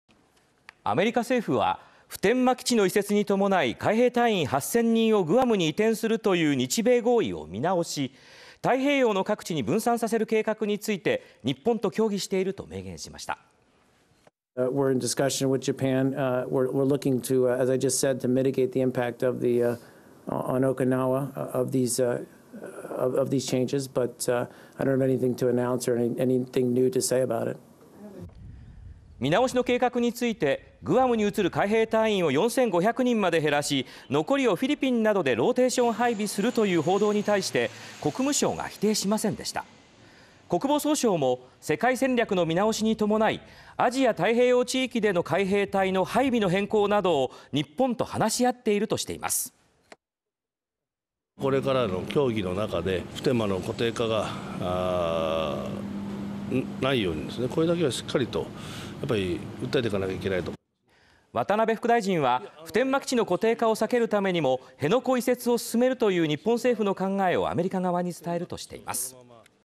グアム移転見直し協議 沖縄海兵隊員8000人（12/02/04）
アメリカ政府は3日、普天間基地の移設に伴い、海兵隊員8000人をグアムにまとめて移転するという日米合意を見直し、太平洋の各地に分散させる計画について「日本と協議している」と明言しました。